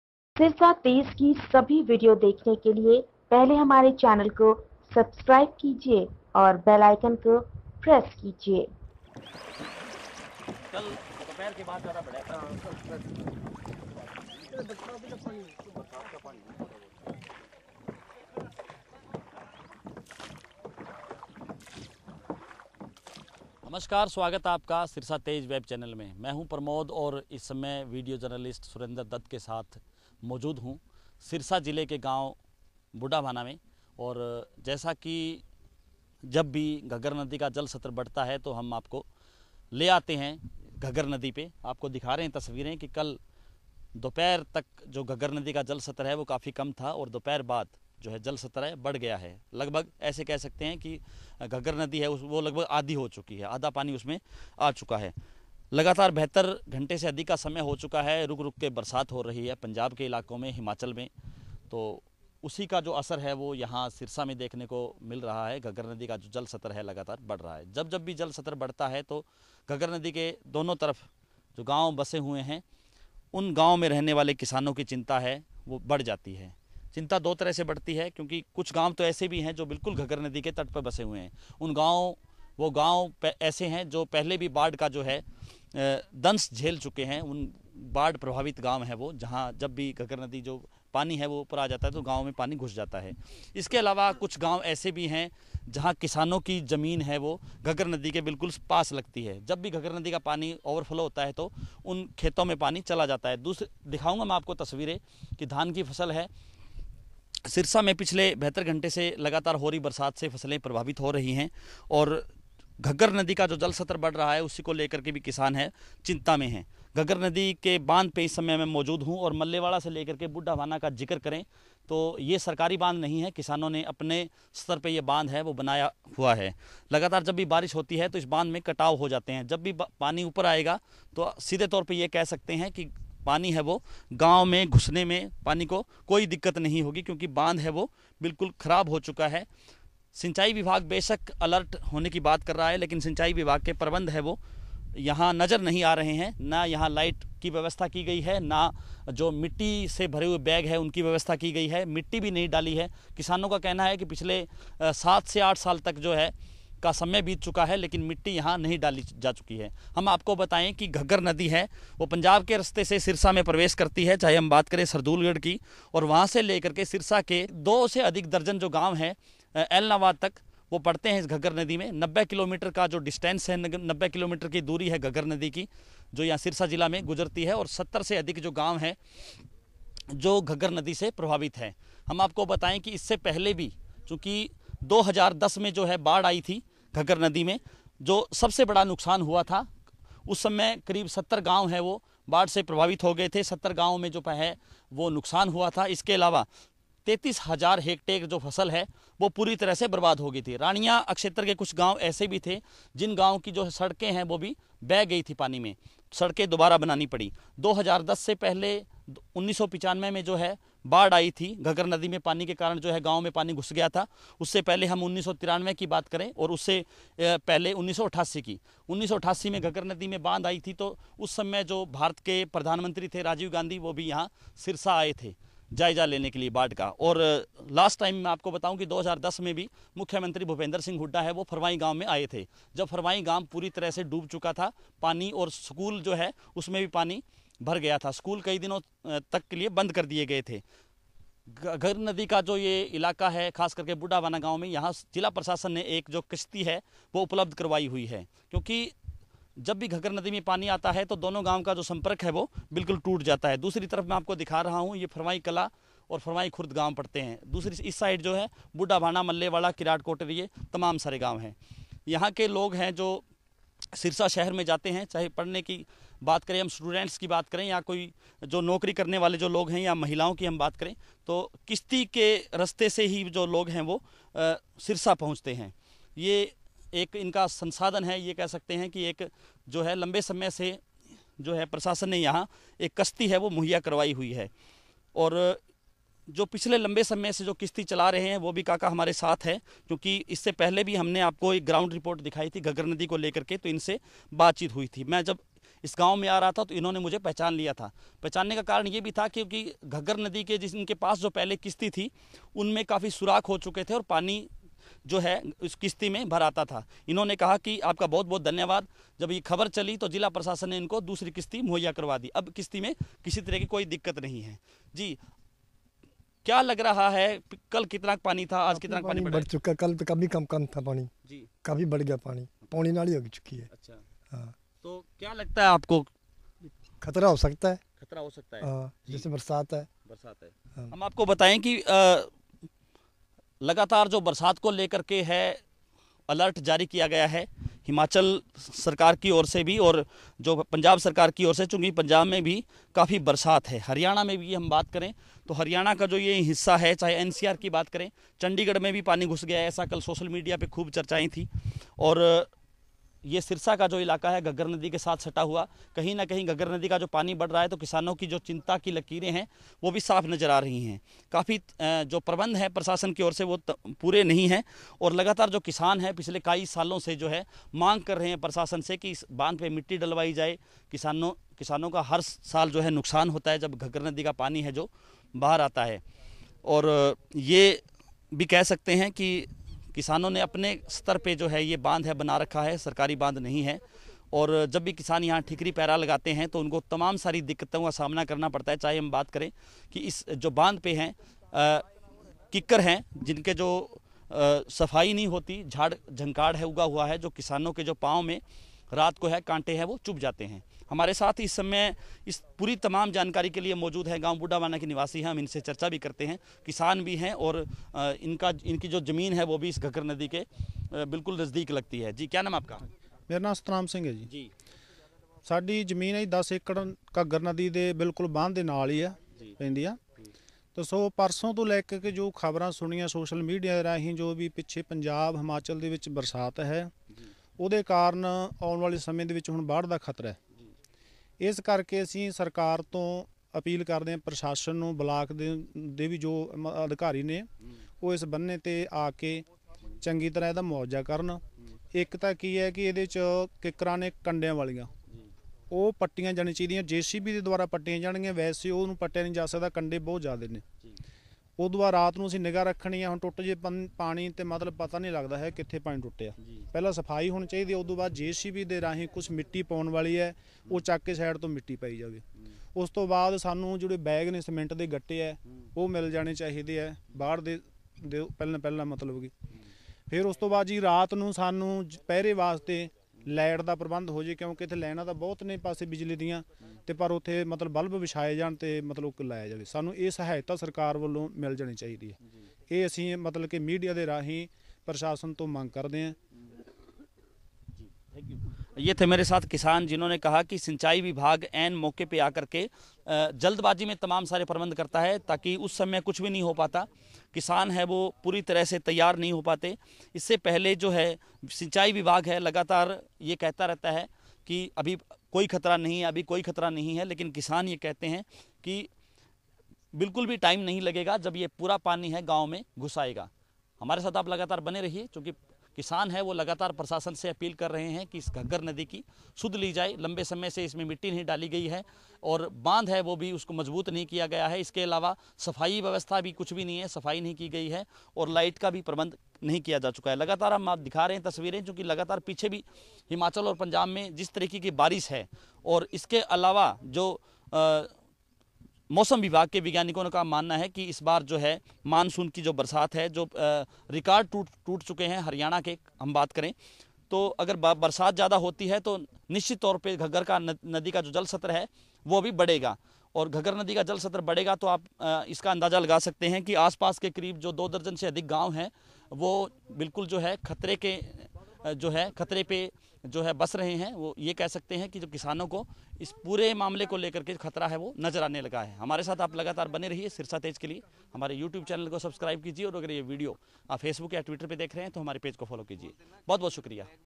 घग्घर नदी में बढ़ा पानी का जलस्तर, किनारे बसे गांवों में 'चिंता'
हरियाणा के सिरसा जिले के करीब 2 दर्जन से अधिक गांवों से होकर गुजरती है घग्गर नदी, पंजाब और हिमाचल में लगातार बरसात से घग्गर में बढ़ा पानी का जलस्तर, किसानों की चिंता बढ़ी, गांवों में मंडराने लगा बाढ़ खतरा, बरसात से घग्गर में आएगा पानी, देखिये सिरसा तेज़ की ये रिपोर्ट प्रमोद कुमार के साथ वीडियो जर्नलिस्ट सुरेंदर दत्त।